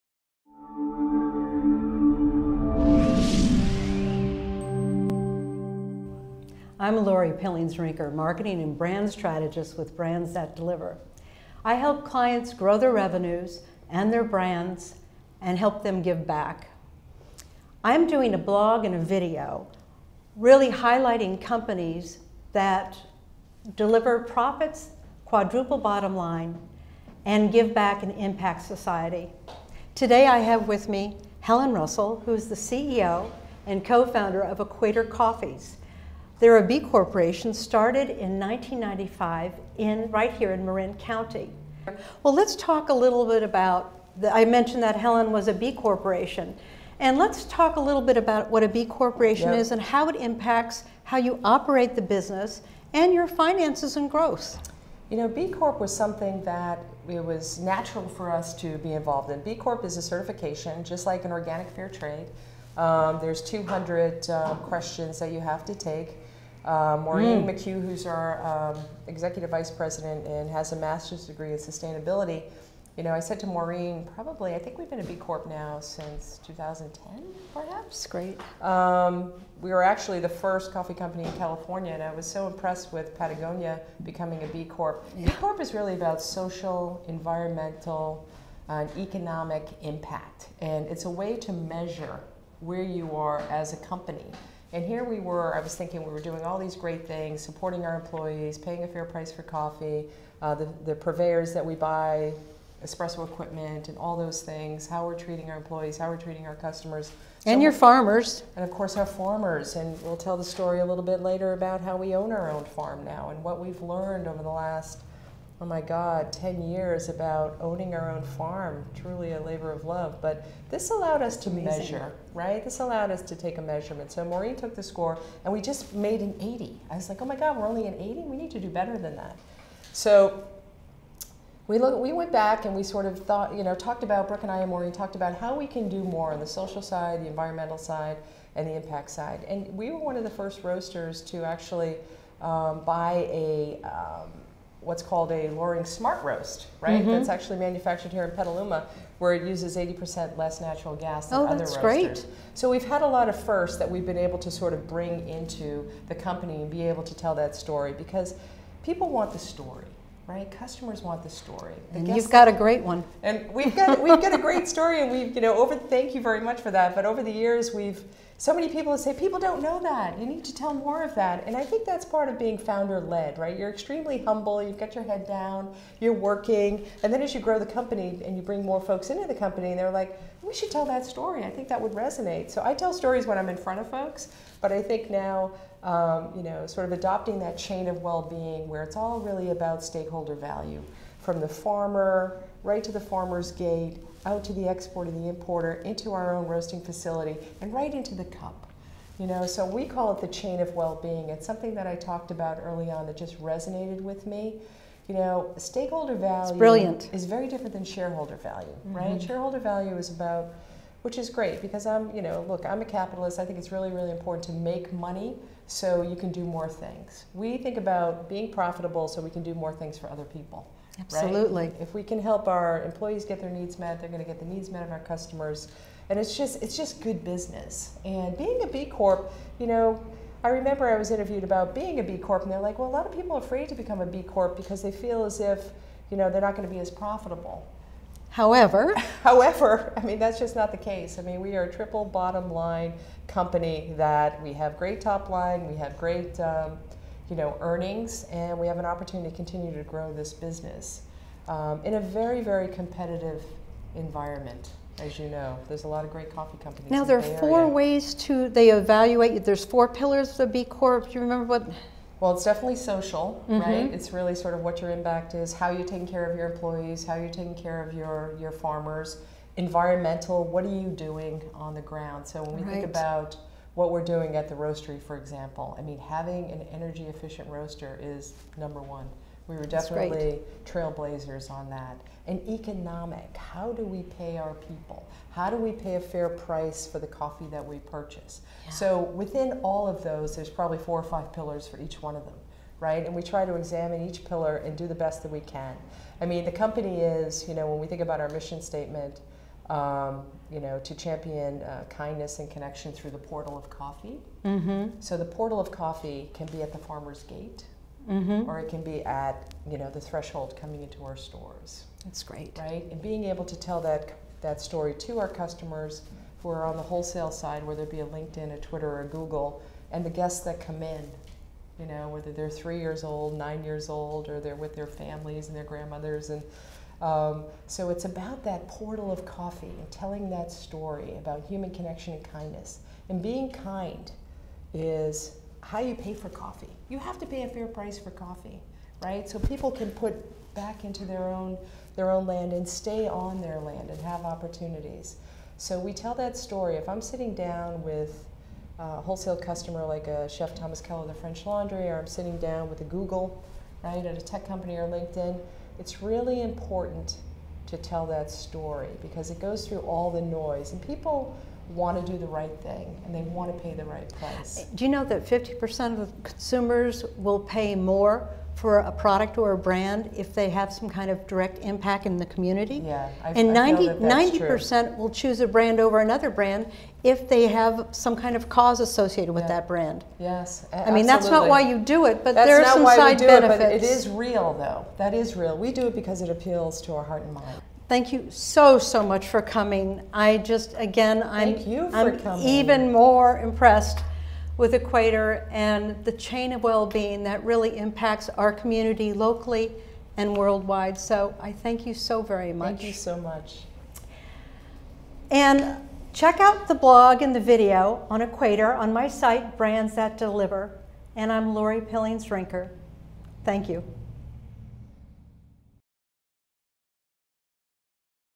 BCorp: Equator Coffees Interview with Helen Russell and Laurie Rinker
Helen Russell of Equator Coffees talks to Laurie Rinker about B-Corp's. Learn about
* B-Corps
* What is it to be one: why it just made sense
* The four pillars: Governance, Workers Community and Customers
*  B-Corps should be profitable
* Why people choose your products over others